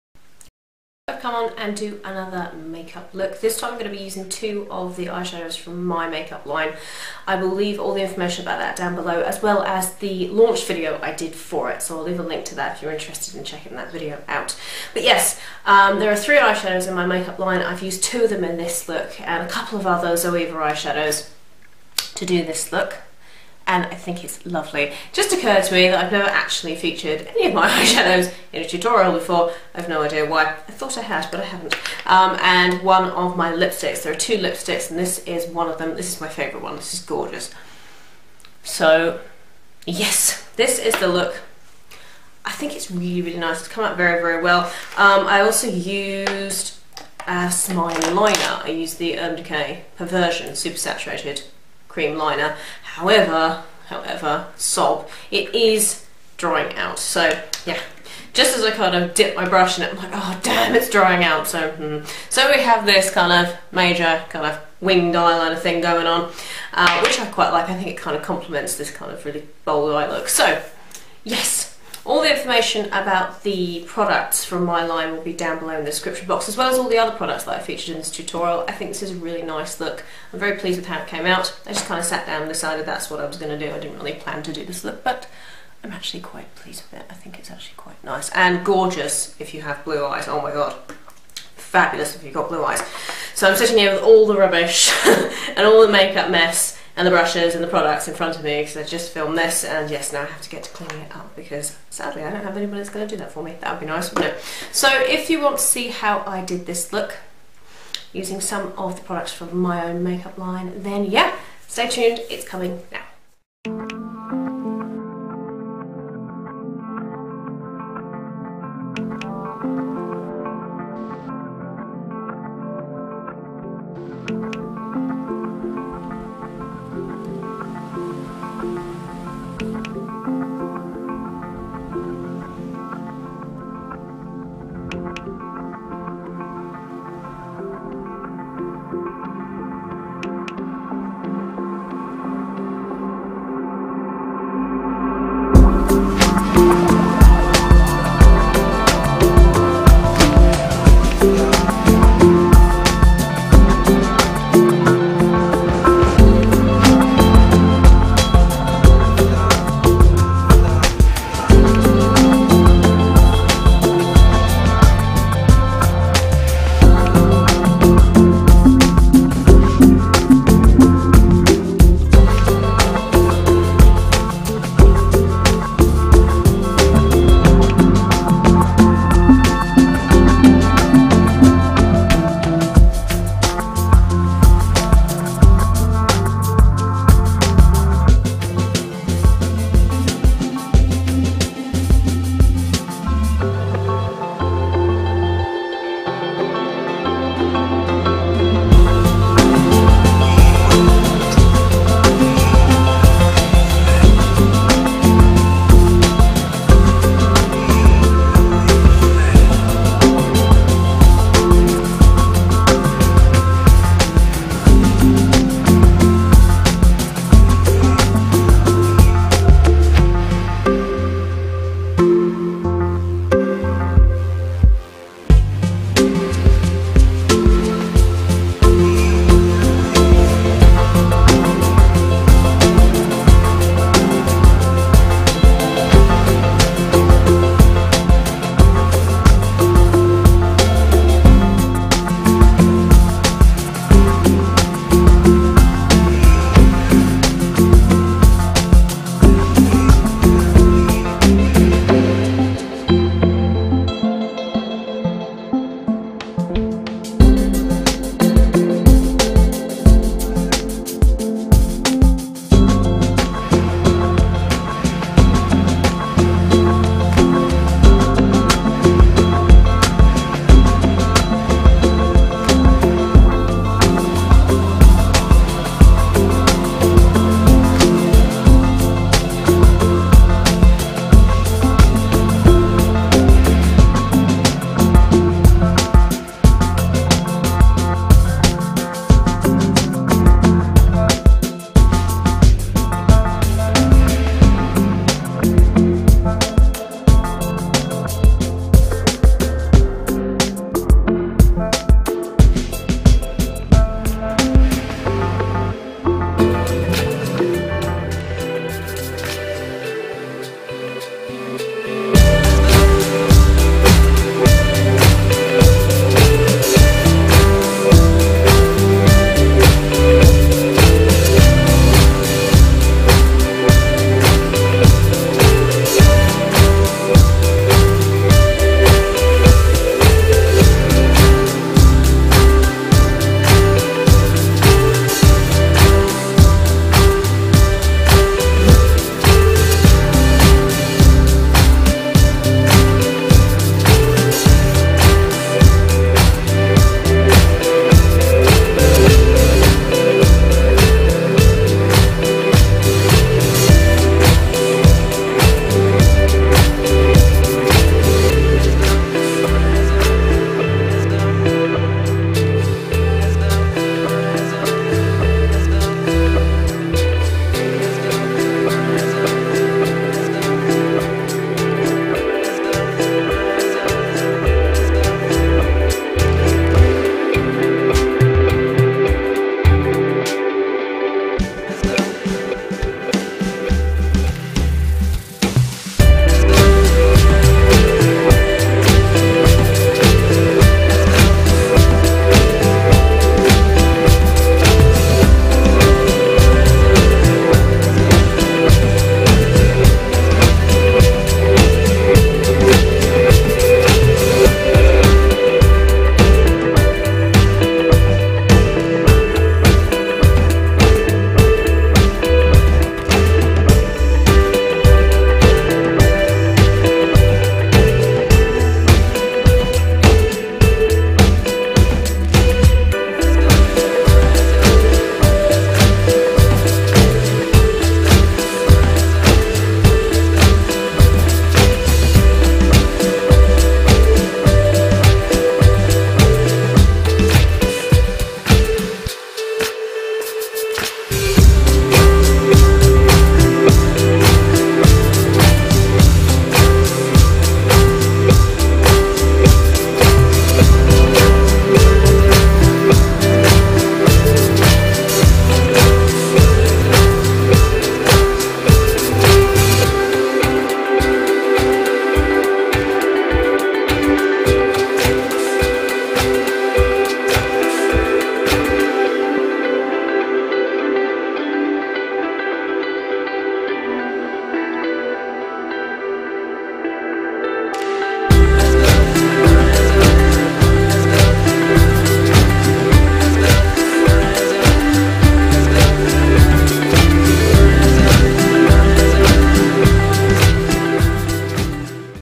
Warm & Bold Makeup ft. My Makeup Line!
A bold, warm makeup tutorial using a few of the products from my own makeup line!

Products Used:
* Wild Kat Makeup Limerence Eye Shadow

* Urban Decay Naked Basics 2 - Skimp e/s
* Urban Decay Super Saturated Cream Liner - Perversion
* Max Factor Masterpiece Mascara
* Glo & Ray Skybreaker Liner - Burgundy
* Too Faced Endless Summer Bronzer

* Laura Mercier Illuminating Powder - Stardust
* Wild Kat Makeup Vespertine Lipstick

☟ BRUSHES I Love. . ☛

- NANSHY -
Masterful Collection:

- SIGMA -
 Make Me Classy Kit

- ZOEVA -
Classic Brush Set

- U SPICY -

|| More Wild Kat Makeup Videos! ||